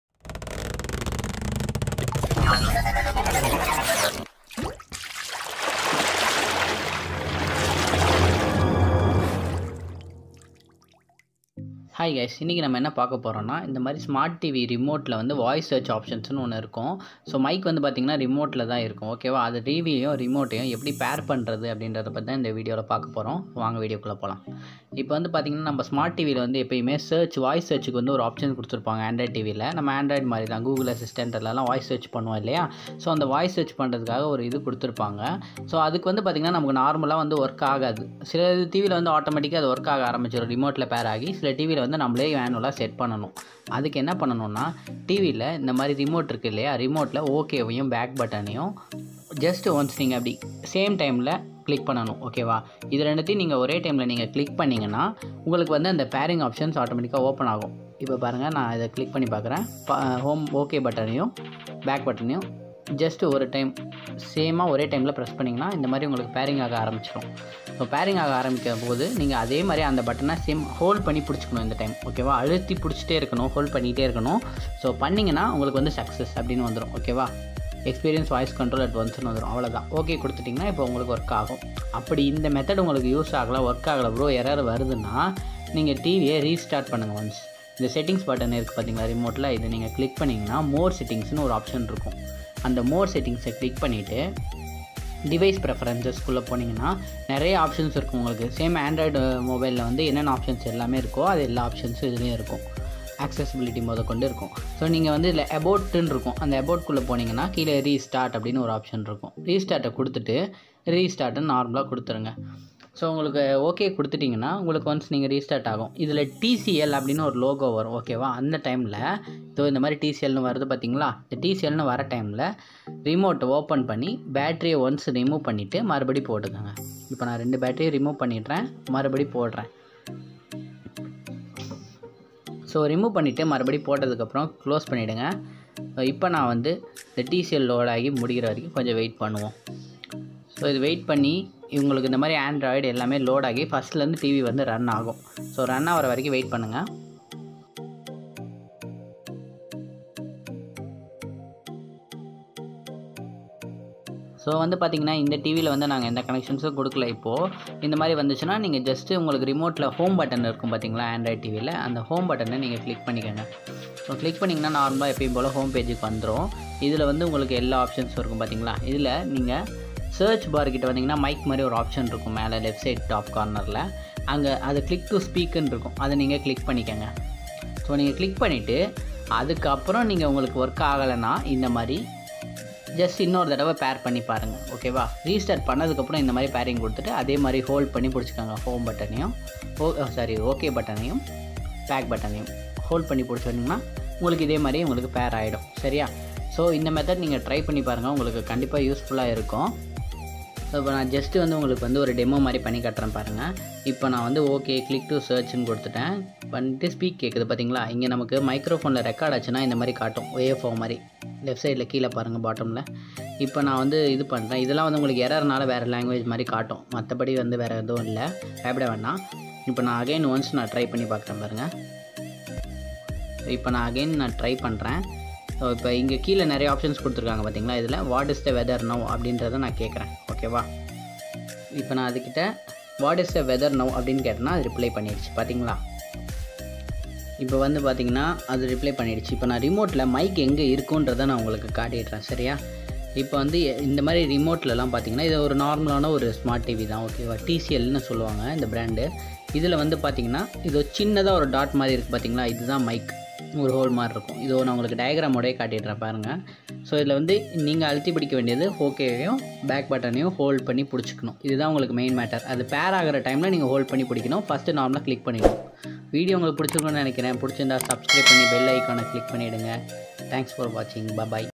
How To Activate Voice Search In any Android /Smart TV Through Simple Steps in Tamil - How To?
How To Activate Google Voice Search in Smart Tv Video in Tamil?

In this video, we are activate voice search in your android tv or remote pairing with few easy steps.

Do Watch our video fully for easy and step by step methods.